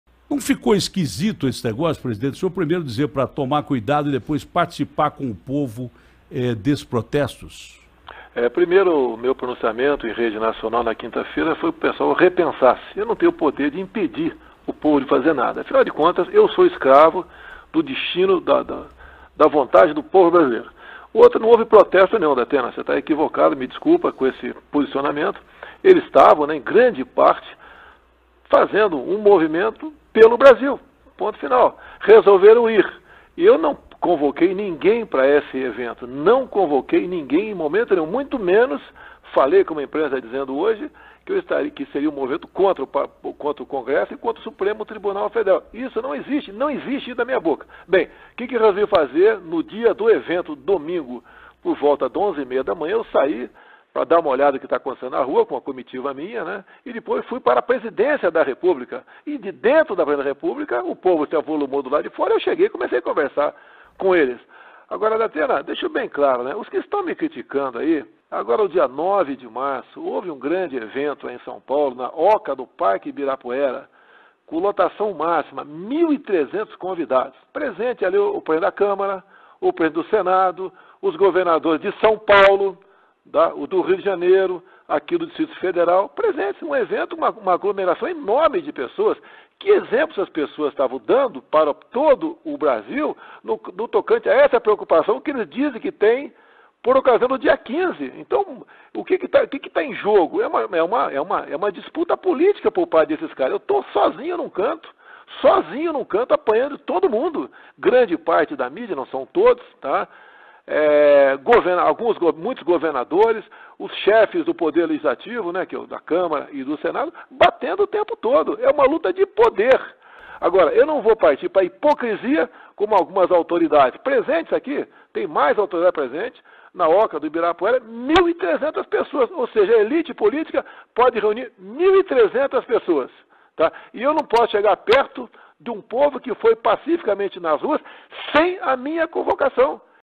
Bolsonaro fala a Datena sobre atos do dia 15/03
Em entrevista a Datena, Presidente Jair Bolsonaro fala sobre as manifestações de 15 de março: “Eu não tenho o poder de impedir manifestações. Eu sou escravo da vontade do povo brasileiro”.